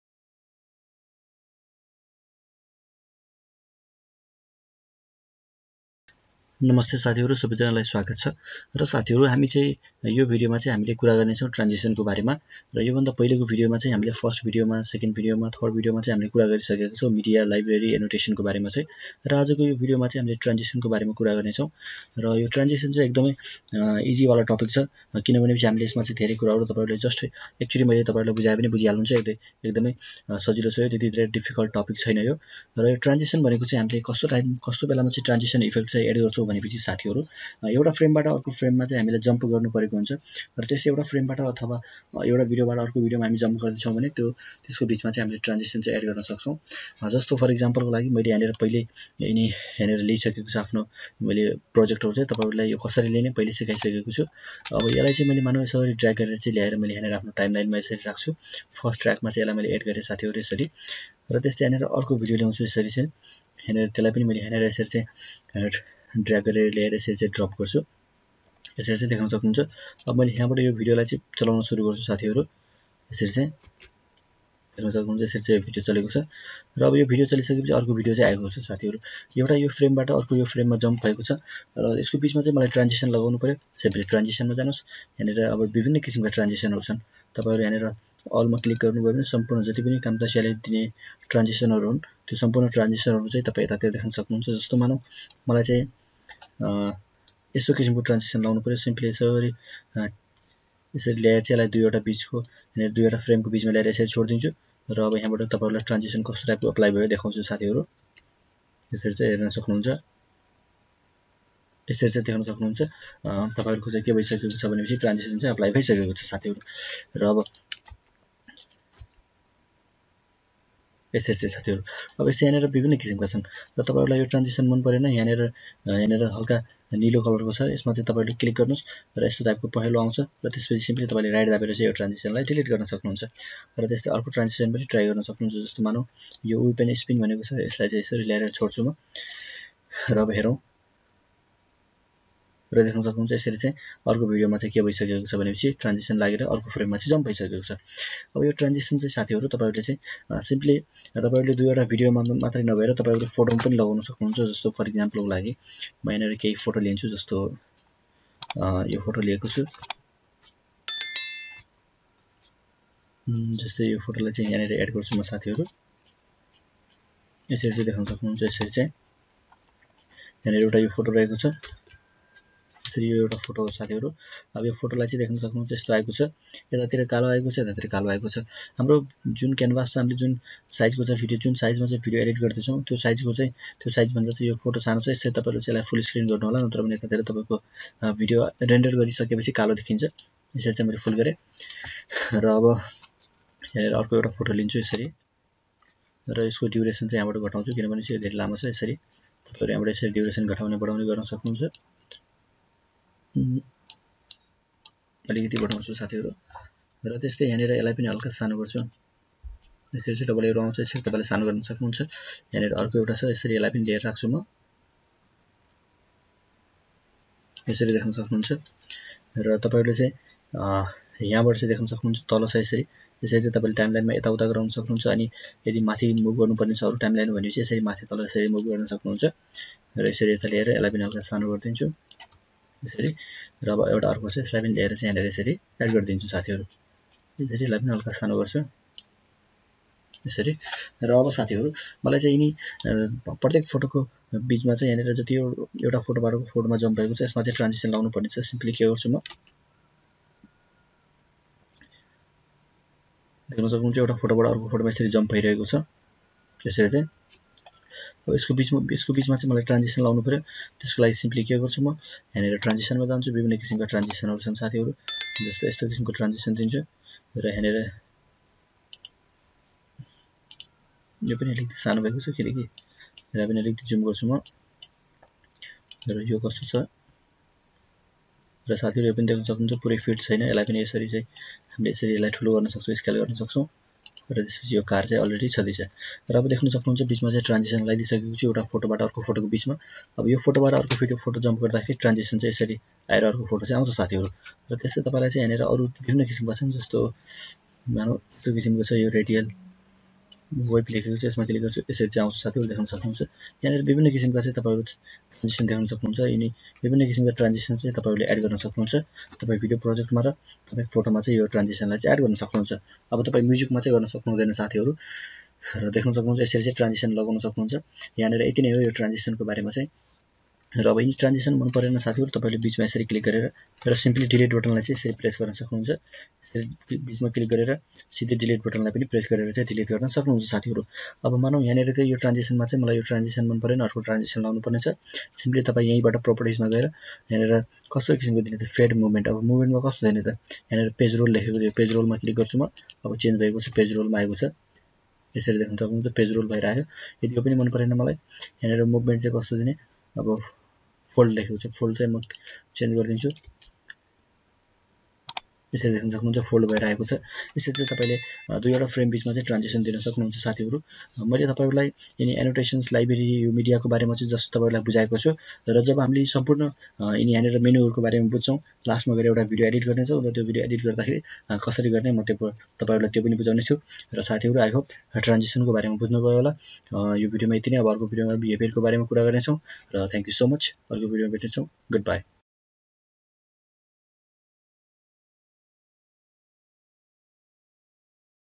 Camtasia Video Editing Tutorial in Nepali | Transitions menu in camtasia 2019 | Part 4
Hi friends in this video we are going to learn video editing in camtasisa software in nepali this is the fourth part of video editing tutorial if you want to learn complete video editing on Camtasia 2019 software So lets get start_______________________